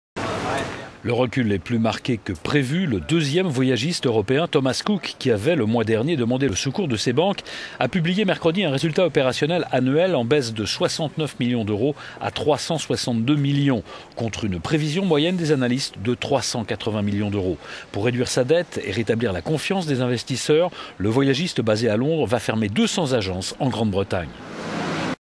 Le voyagiste Thomas Cook va fermer 200 agences
Le recul est plus marqué qu'attendu : le 2ème voyagiste européen Thomas Cook qui avait le mois dernier demandé le secours de ses banques, a publié mercredi un résultat opérationnel annuel en baisse de 69 millions d'euros à 362 millions contre une prévision moyenne des analystes de 380 millions d'euros. Pour réduire sa dette et rétablir la confiance des investisseurs, le voyagiste basé à Londres va fermer 200 agences en Grande Bretagne.